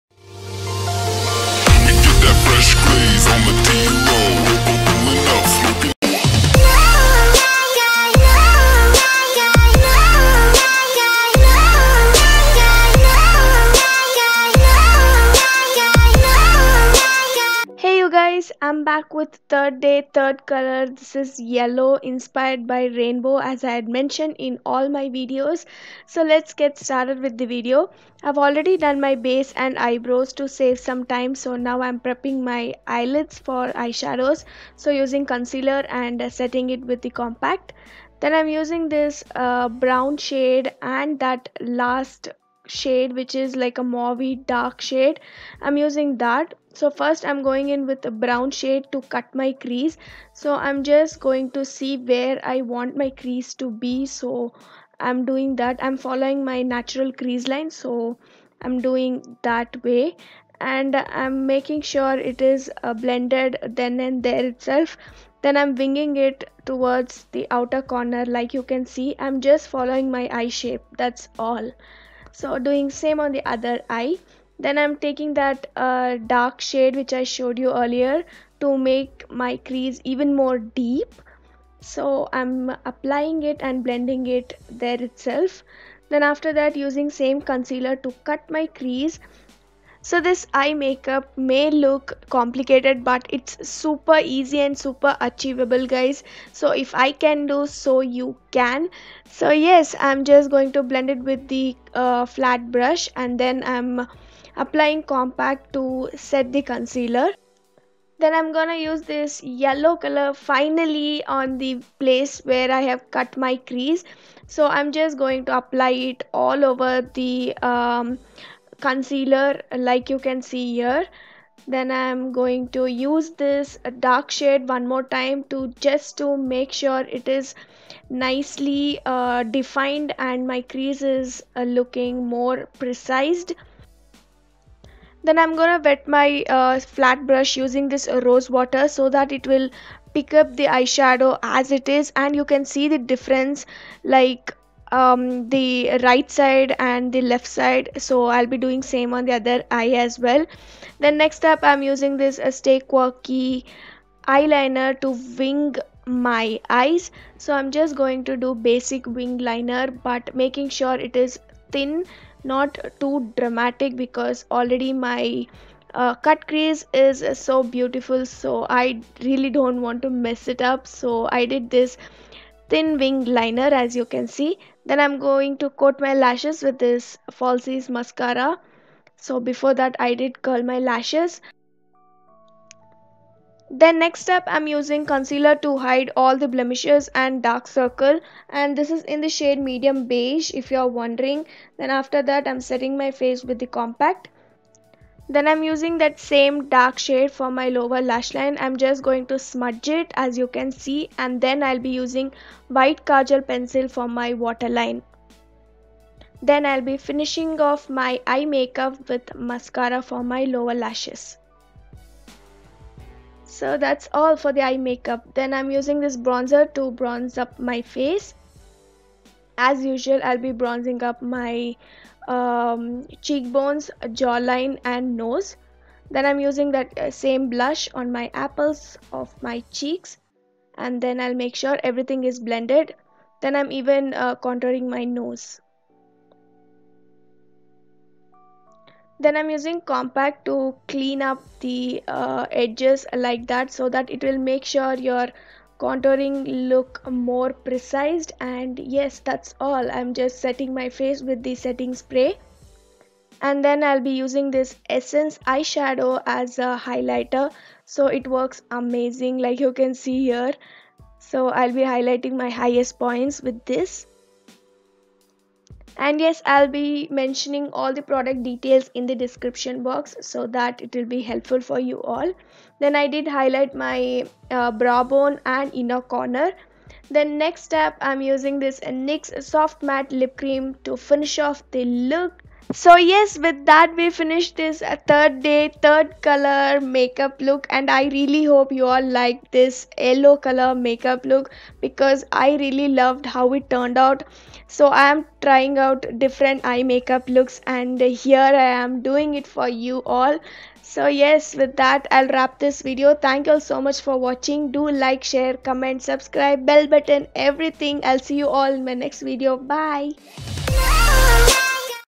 💛YELLOW MAKEUP TUTORIAL🍋 | RAINBOW COLORS INSPIRED MAKEUP LOOK | VIBGYOR 🌈 | Pavithra iyer
Hi Everyone :) Today am Sharing MY THIRD THEME BASED MAKEUP LOOK WHICH IS INSPIRED BY RAINBOW🌈COLORS VIBGYOR :)SO AM DOING 7DAYS7COLORS CHALLENGE & DIZ IS THIRD MAKEUP WITH YELLOW COLOR🍋 & I WILL BE POSTING DIZ FULL WEEK DIFFERENT MAKEUP LOOKS WITH PARTICULAR COLOR :) SO DON'T FORGET TO SUBSCRIBE SO DAT YO WON'T MISS ANY VIDEOS DIZ WEEK :) Hope yo all like this attempt of mine & it will be helpful :) 💛

PRODUCTS:-
ALOVERA GEL
STUDIO WEST FOUNDATION IN 03 GOLDEN
MAYBELLINE FASHION BROW IN GREY
LA GIRL PRO CONCEAL IN MEDIUM & CREAMY BEIGE
LAKME RADIANCE COMPACT IN CORAL
SIVANNA HD ULTIMATE EYESHADOW PALETTE IN 02
GLAM21 PROFESSIONAL 51 COLOR EYESHADOW PALETTE
STAY QUIRKY BADASS EYELINER
MAYBELLINE FALSIES PUSH DRAMA MASACARA
LAKME EYECONIC KAJAL IN WHITE
MAKEUP REVOLUTION ICONIC IN SMOULDER
KISS BEAUTY MAKEUP MIST
ESSENCE EYESHADOW IN 14 PEACH BEACH
NYX SOFT MATTE LIP CREAM IN SAO PAULO